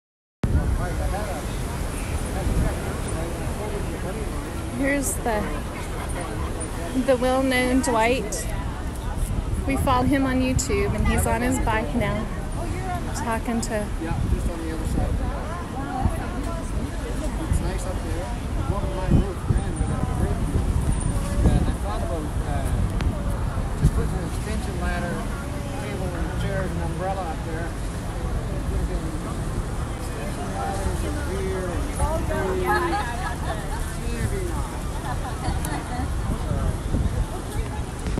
Stacey Ann a friend from Arkansas shot this. Me talking to two viewers from Toronto
My bike is fixed!